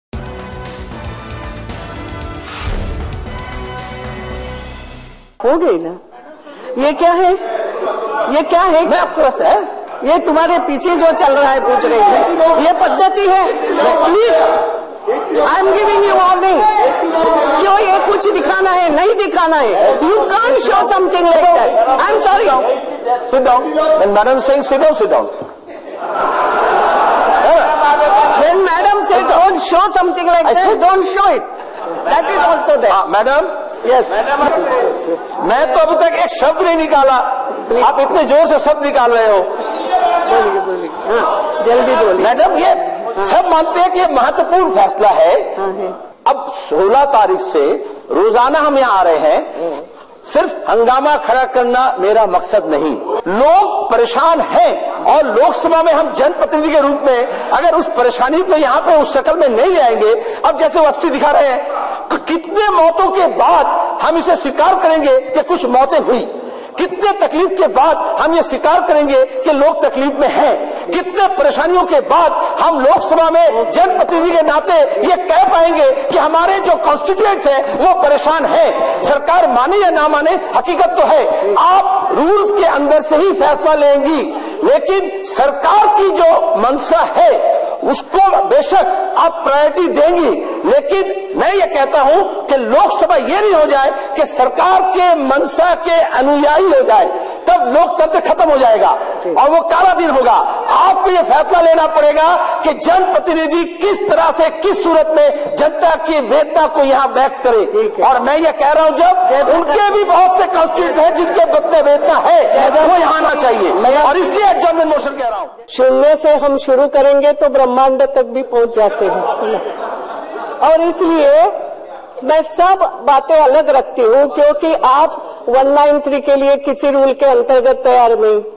Mohammad Salim Makes Fun On Lok Sabha Speaker Sumitra Mahajan | Demonetisation Debate | Mango News
Watch Mohammad Salim Makes Fun On Lok Sabha Speaker Sumitra Mahajan and Demands for Debate on Demonetisation.